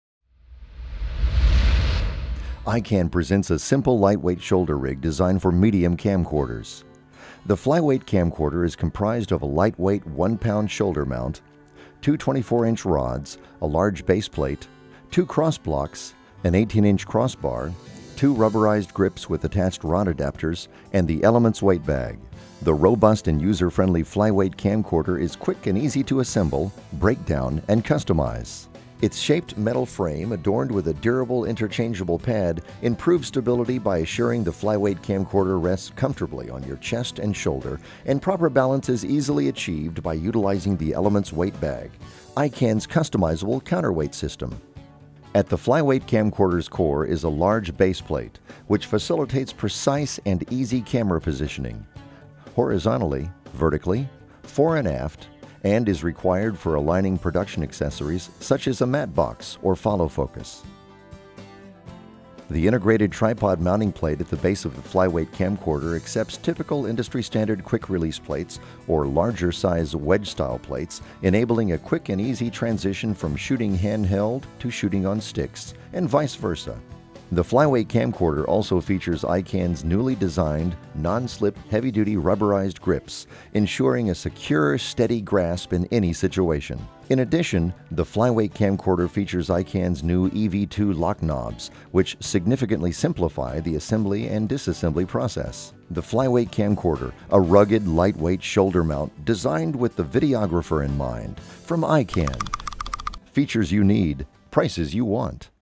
Flyweight Camcorder (with Counter Balance Weight Bag) - ikan
Flyweight Camcorder

ikan proudly presents the robust, user friendly Flyweight Camcorder. Key components are preassembled for the video professional's convenience. Its enhanced lightweight shoulder pad system assures the Flyweight Camcorder rests comfortably on your chest and shoulder. The rig features ikan's prestigious, adjustable base plate providing limitless shot options. The rig is designed to easily stack components customizing it to adapt to the video professional's needs. With the new heavy duty rubberized grips you can truly get a grip!  The Flyweight Camcorder can be easily accessorized with a LANC controller for added shot capabilities.

for more information on this rig.